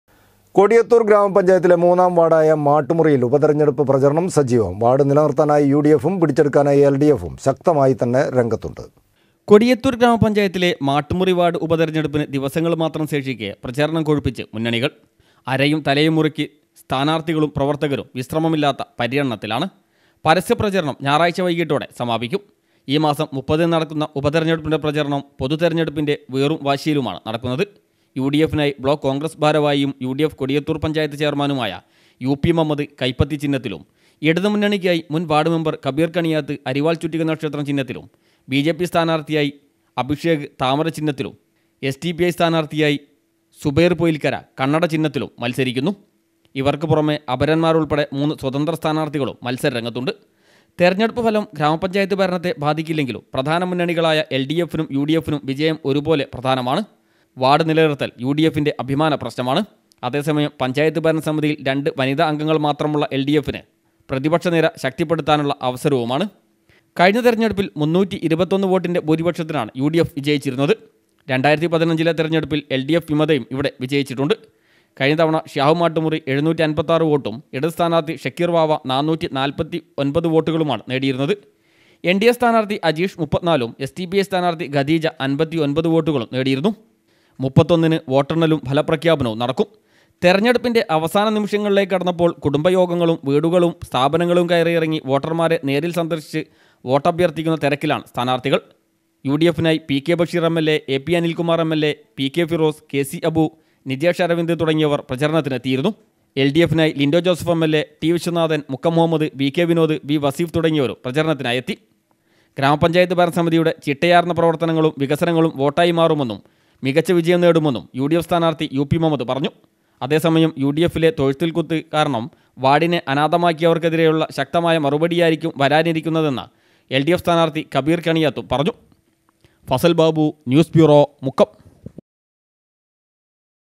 കൊടിയത്തൂർ ഗ്രാമപഞ്ചായത്തിലെ മുന്നാം വാർഡായ മാട്ടു മുറിയിൽ ഉപതെരഞ്ഞെടുപ്പ് പ്രചരണം സജീവം.
കൊടിയത്തൂർ ഗ്രാമപഞ്ചായത്തിലെ മുന്നാം വാർഡായ മാട്ടു മുറിയിൽ ഉപതെരഞ്ഞെടുപ്പ് പ്രചരണം സജീവം. വാർഡ് നിലനിർത്താനായി യുഡിഎഫും പിടിച്ചെടുക്കാനായി എൽഡിഎഫും ശക്തമായി തന്നെ രംഗത്തുണ്ട്